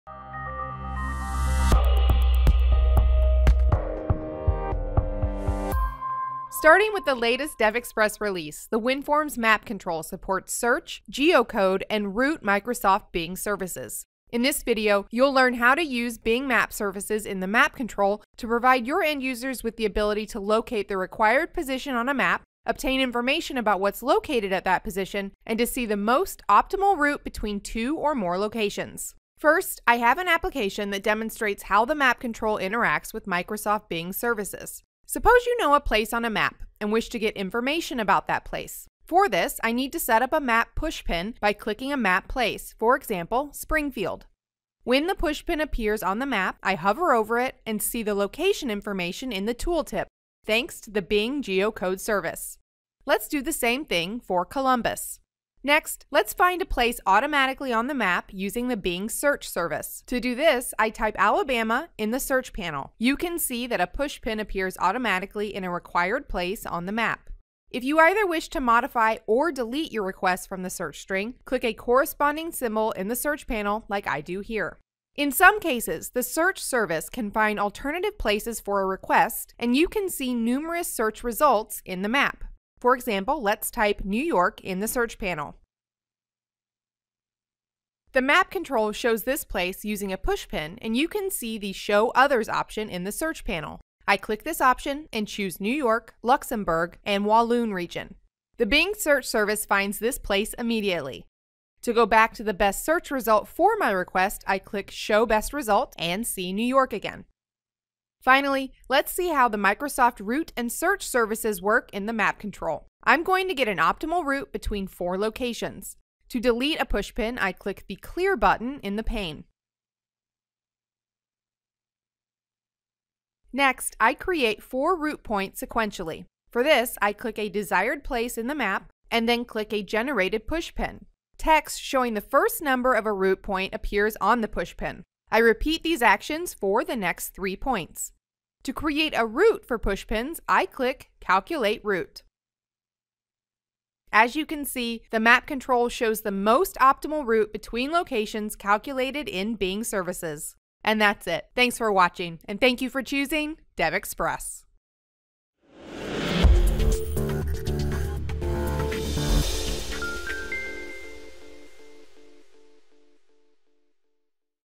DevExpress WinForms: Map Searching and Routing with Bing Services
This video shows how to use online Microsoft Bing Geocode, Bing Route, and Bing Search Services in the map control to locate any place on a map, see the detailed information for this place or specify a route consisting of two or more locations.